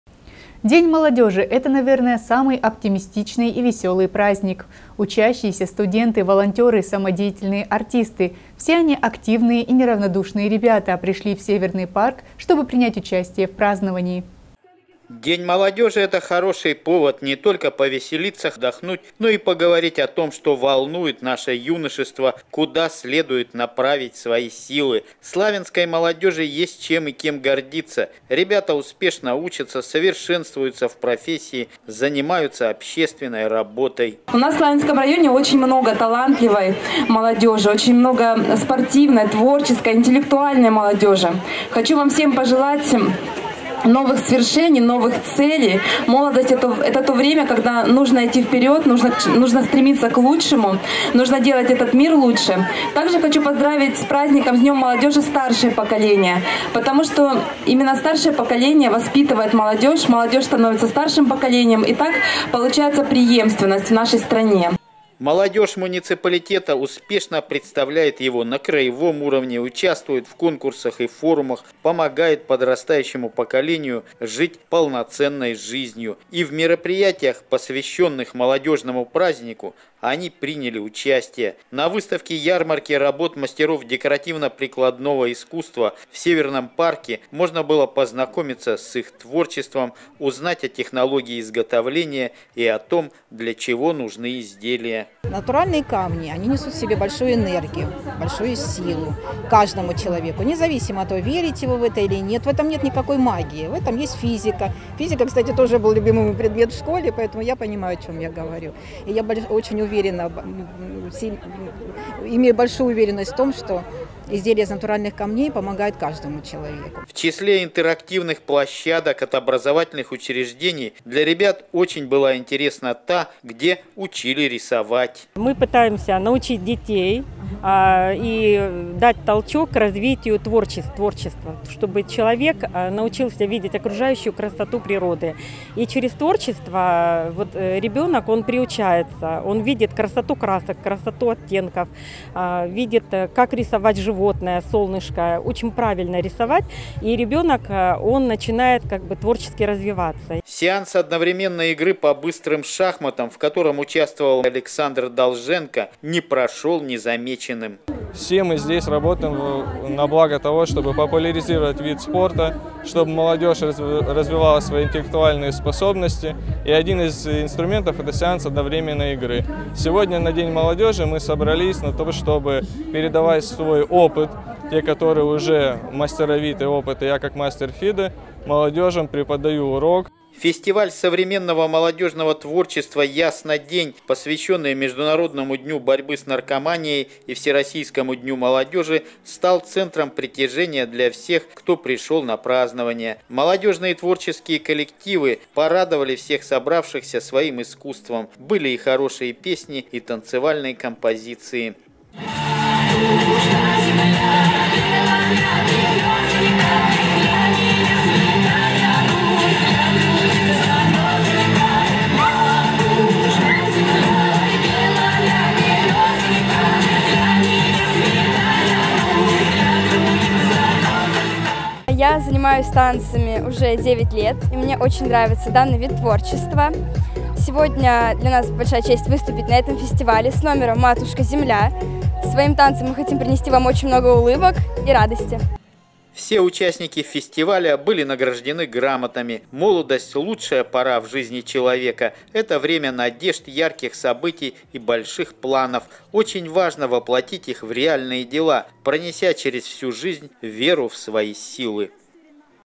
Праздник молодых
День молодежи — это, наверное, самый оптимистичный и веселый праздник. Учащиеся, студенты, волонтеры, самодеятельные артисты – все они активные и неравнодушные ребята - пришли в Северный парк, чтобы принять участие в праздновании